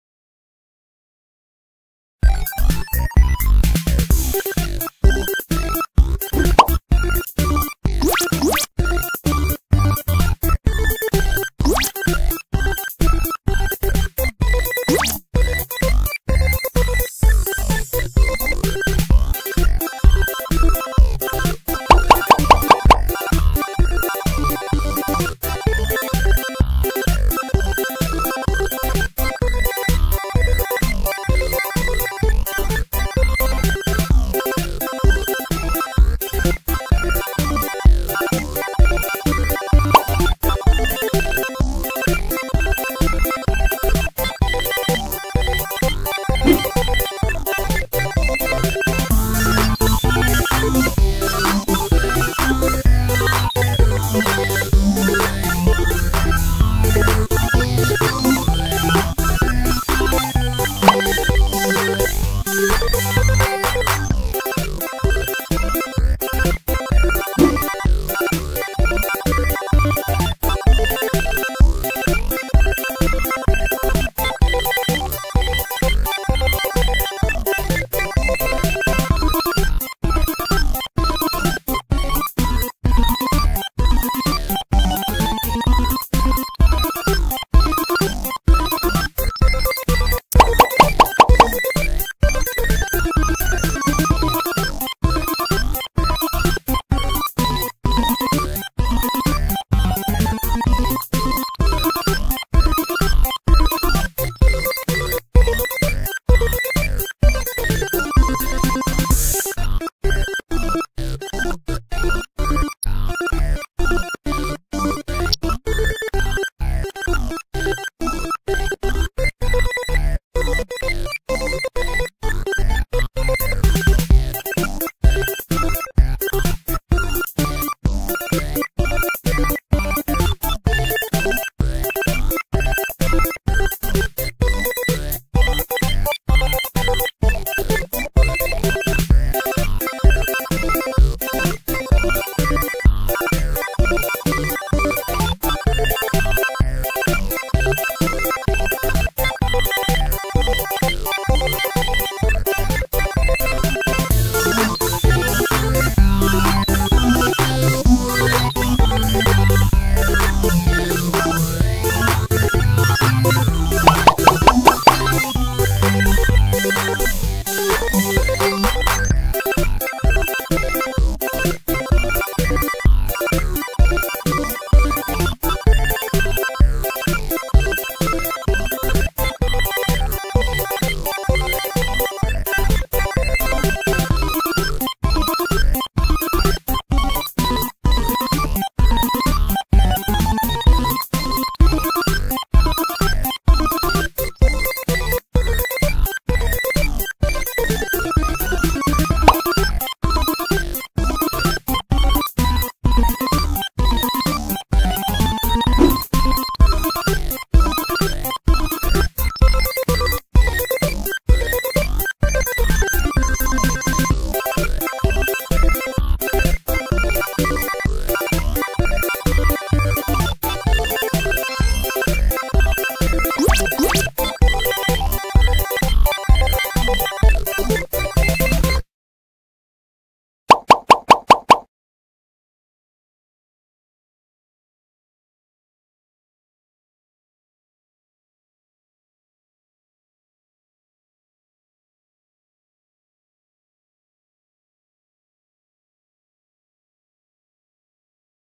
Easy & Cute! Learn How to Draw a Fat Dog in Kawaii Chibi Style
In this YouTube tutorial, we will guide you through the process of drawing an adorable and chubby fat dog in a simple and cute kawaii style. Whether you are a beginner or an experienced artist, this step-by-step guide will help you create a lovable fat dog with ease. Grab your pencil and paper, and let's get started on this fun and delightful drawing journey!